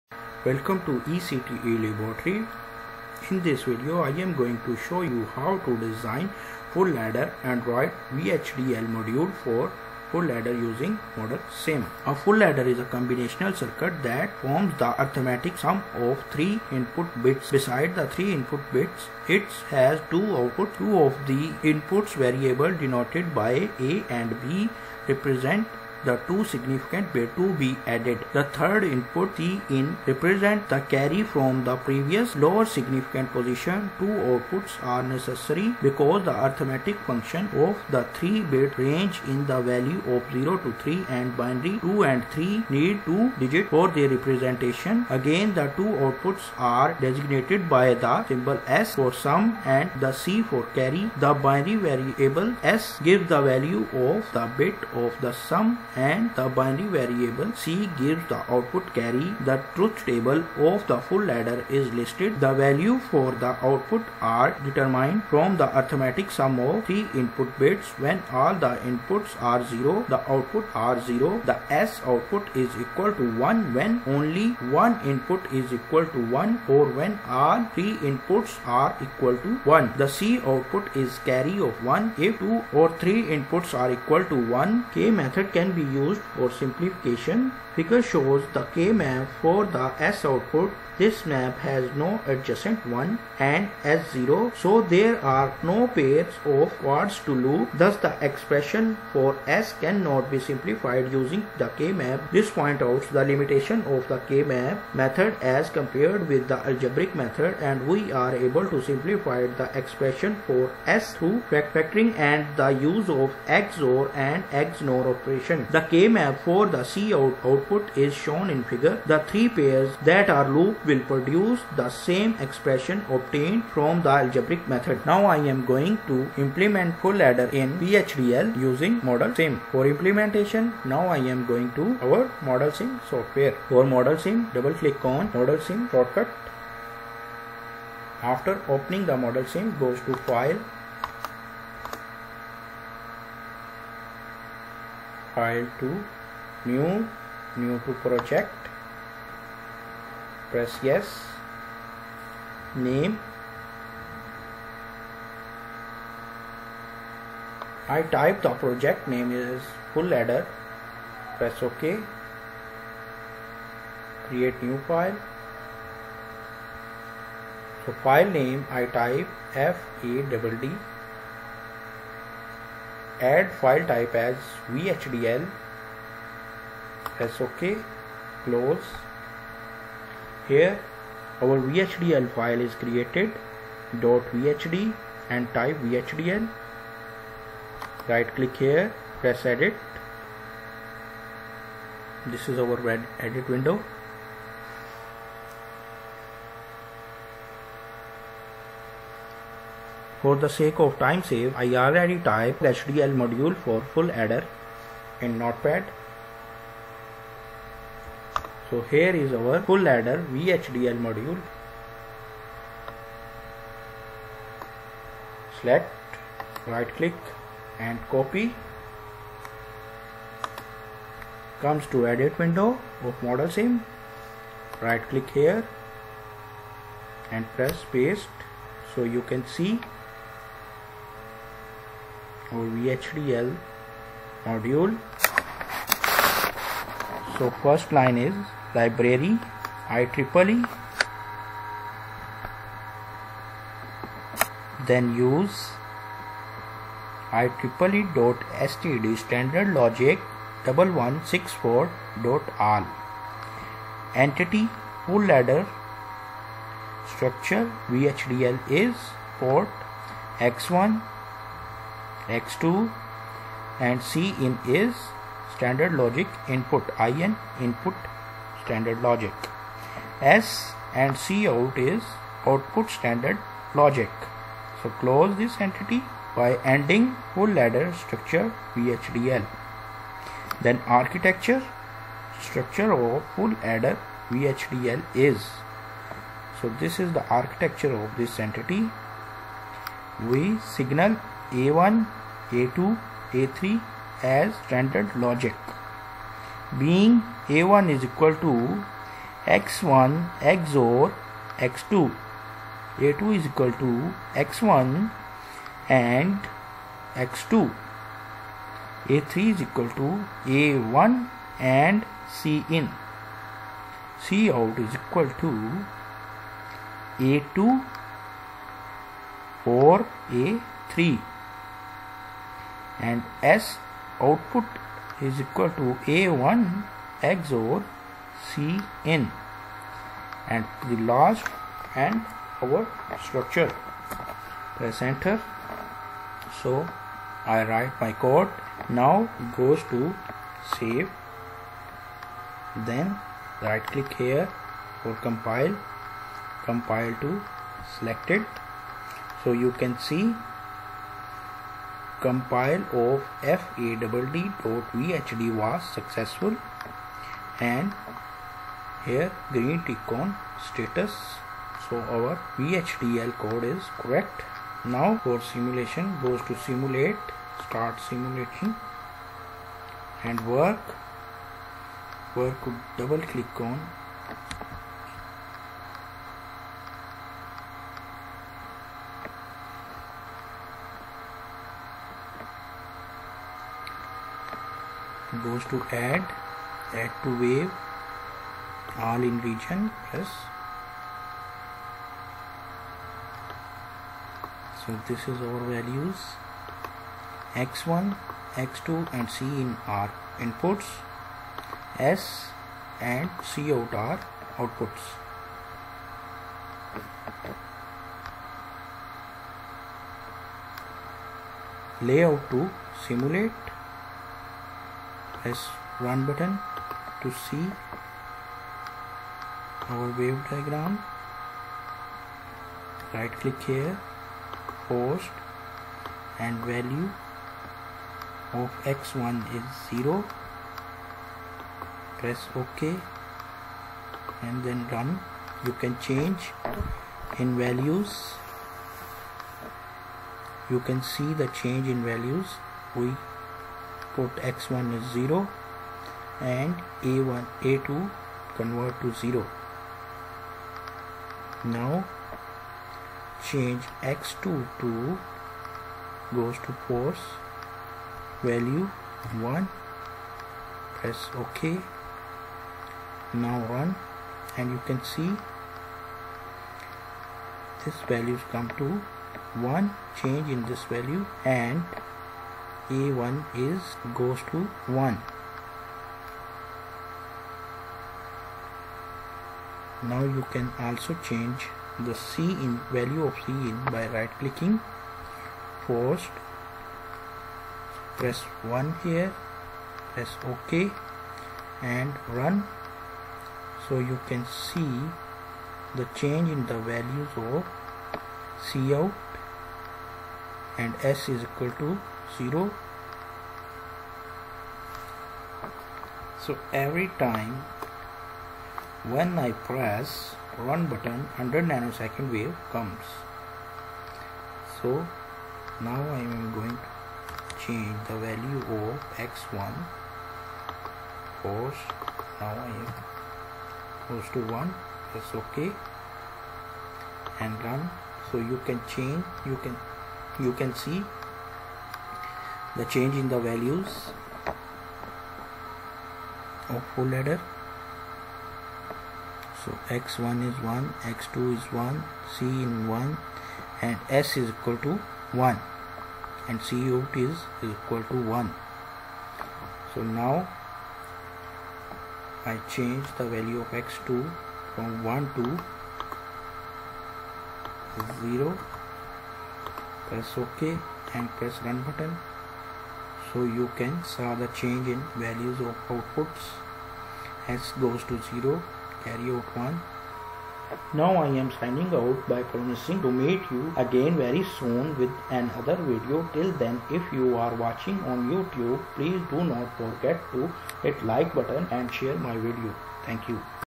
How to Design Full Adder & write VHDL module for Full Adder using ModelSim
After this video, you will be able to.
1. To Write the VHDL module using ModelSim
2. To Write Full Adder VHDL module using ModelSim
3. To Write, Compile, and Simulate a VHDL model using ModelSim
4. To write VHDL module for digital circuits at the Combinational logic.
5. To Design a Full Adder.
6. To Simplified the expression for sum and Cout using Karnaugh Map.
7. To understand Full Adder Truth Table